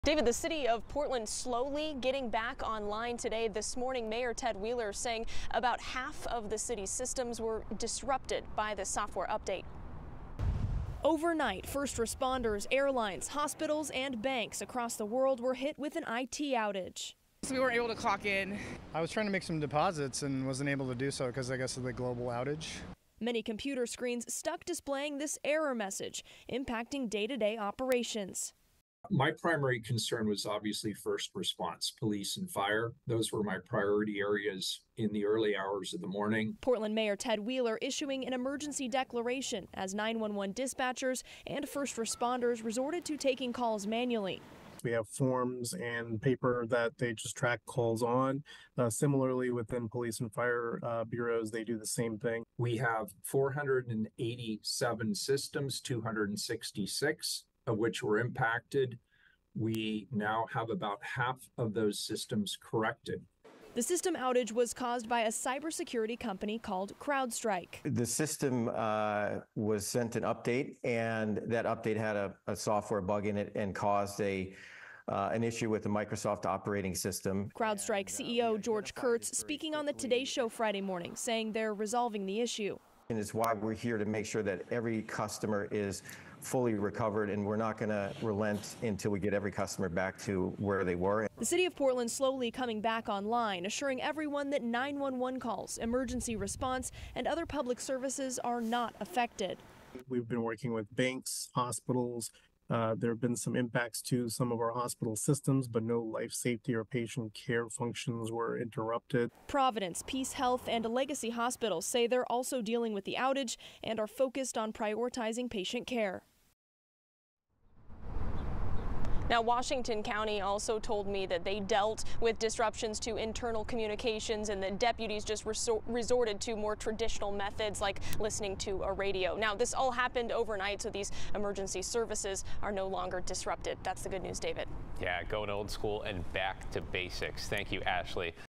Global CrowdStrike outage impacts Portland
Half of the city's systems were disrupted, with Mayor Ted Wheeler declaring a state of emergency.